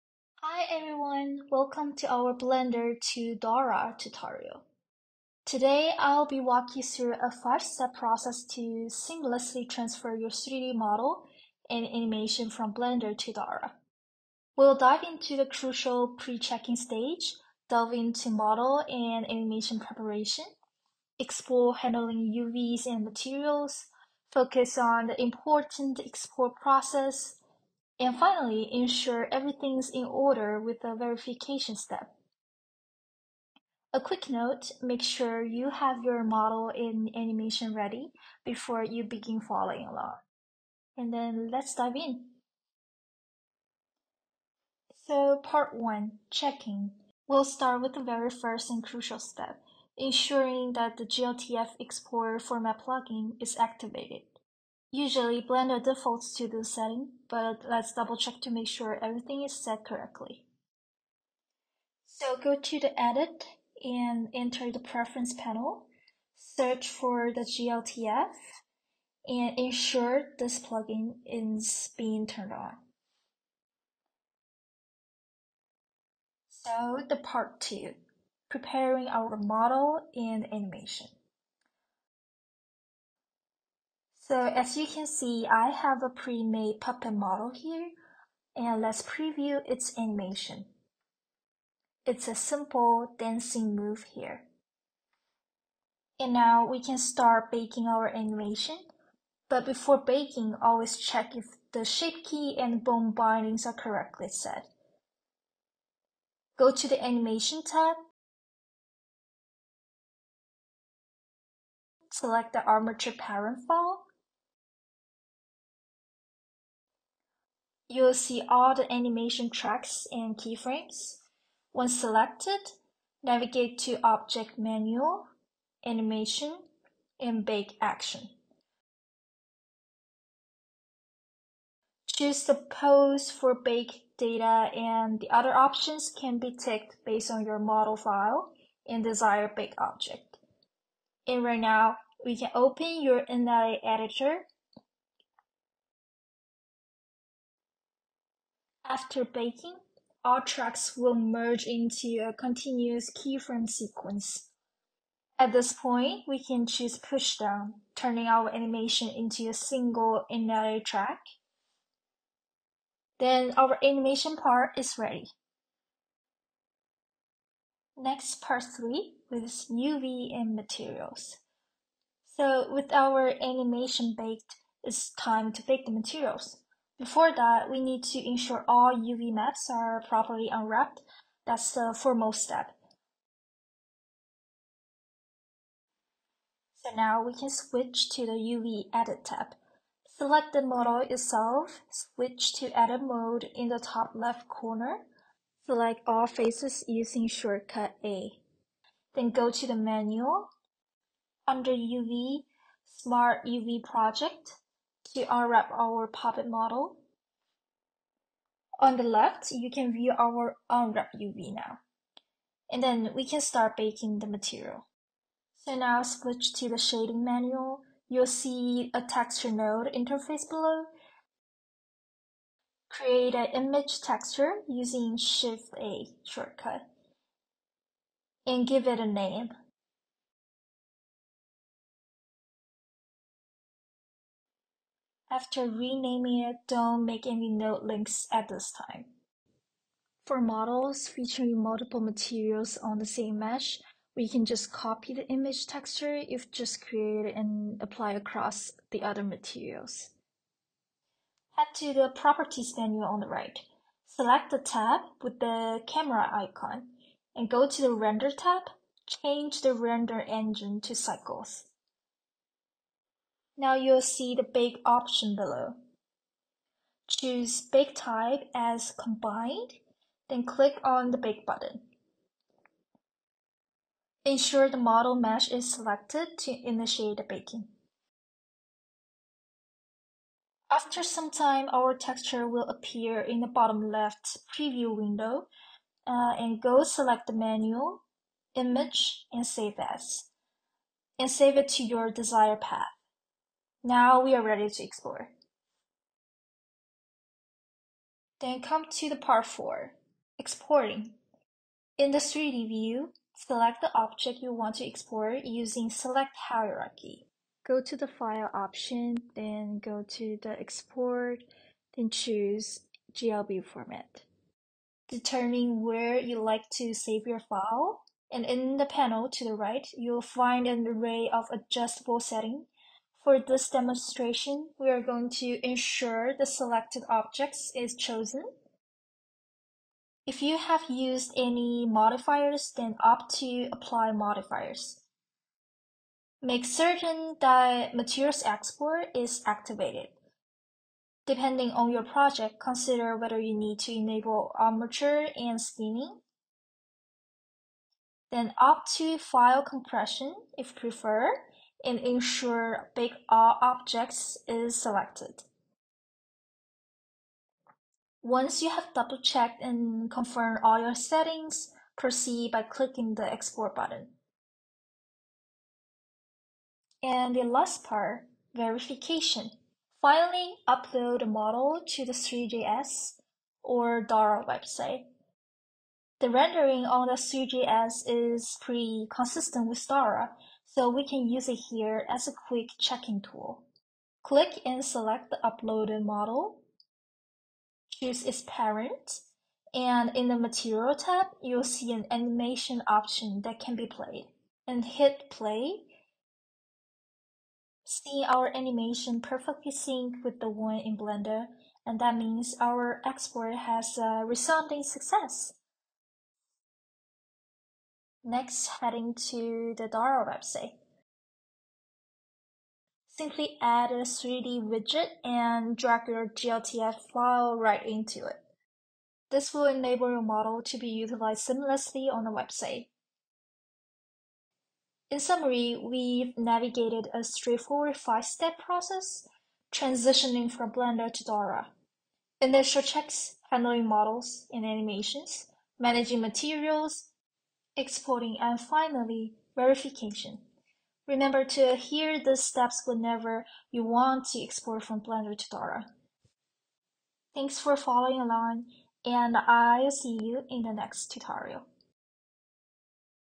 Blender to Dora Tutorial: Transfer your 3D Models and Animations to Websites
In this tutorial, you will learn how to seamlessly transfer your 3D models and animations from Blender to Dora, and create your unique website!

00:00 Intro
00:44 Checking
01:16 Model and Animation
02:52 UV and Materials
04:58 Exporting
06:08 Exporting
07:06 Recap